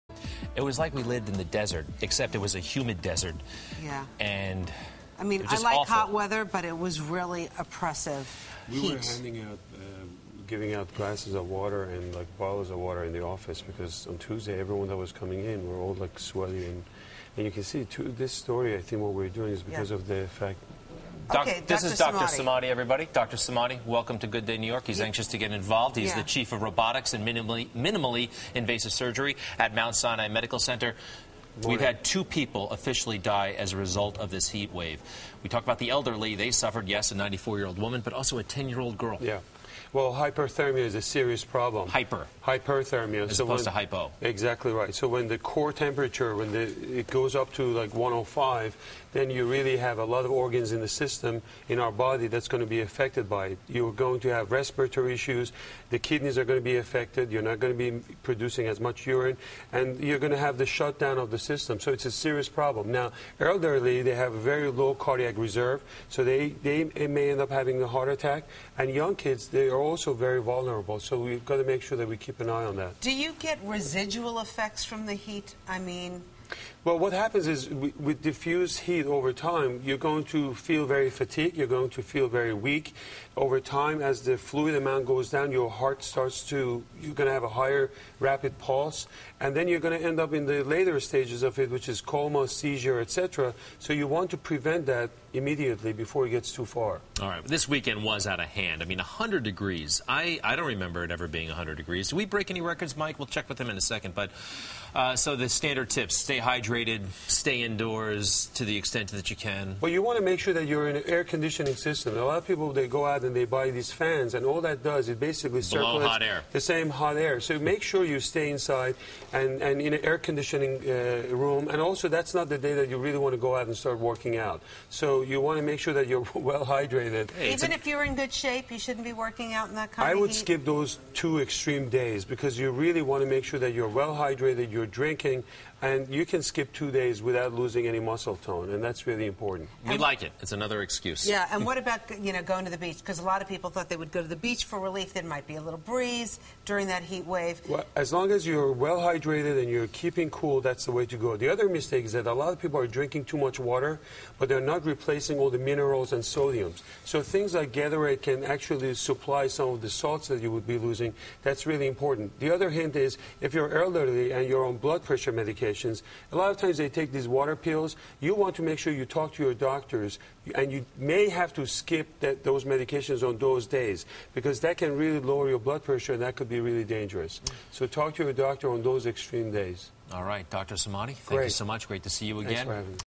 Dr. David Samadi - Hyperthermia Risk and Prevention Tips
Hyperthermia is the general name given to a variety of heat-related illnesses. The condition occurs when the body absorbs or produces more heat and humidity than can be dissipated. Hyperthermia can lead to serious illnesses and possibly death if not treated immediately. Elderly people and children are particularly susceptible to developing hyperthermia in excessively hot and humid weather. To mitigate hypethermia risks, Dr. Samadi, Chairman of Urology and Chief of Robotic Surgery at Lenox Hill Hospital, recommends that people of all ages stay hydrated and remain in an air conditioned environment, when possible. Outdoor exercise in extreme hot temperatures should also be avoided.